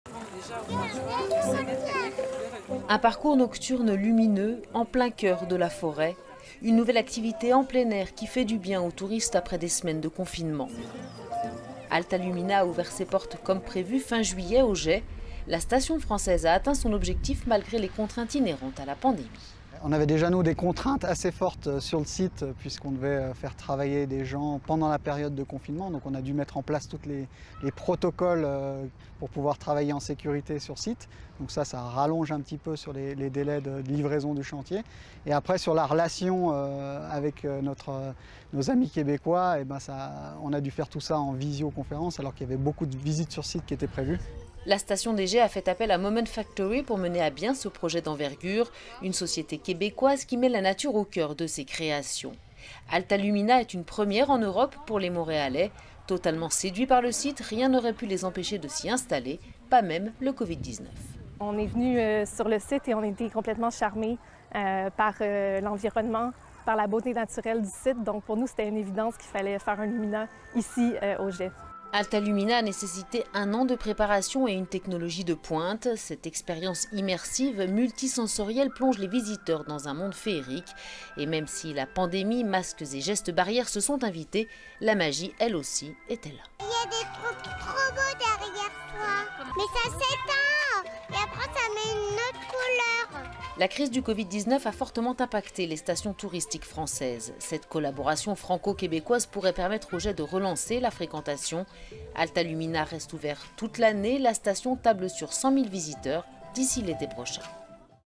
Le projet franco-québécois Alta Lumina voit le jour malgré le Covid-19 : une première en Europe
Malgré le Covid-19, certains projets lancés en amont de la crise sanitaire mondiale ont vu le jour. Le parcours nocturne Alta Lumina, fruit d'une collaboration franco-québécoise, a donc ouvert ses portes comme prévu, aux Gets, en Haute-Savoie. Une première en Europe.